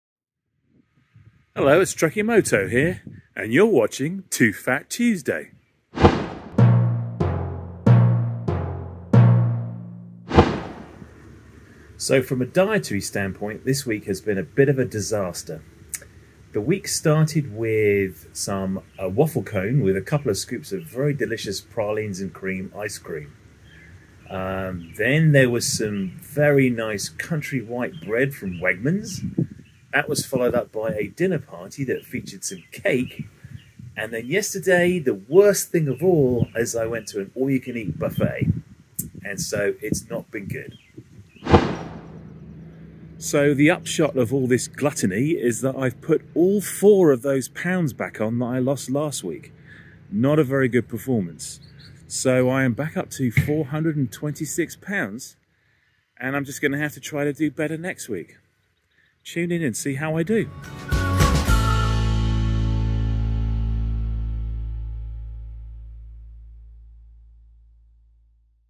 TOO FAT TUESDAY — Week 5
Another week has gone by. How did TrekkieMoto do with his weight?

I wouldn't be able to do this without rewarding myself from time to time, so I buy delicious, low carb chocolate from ChocZero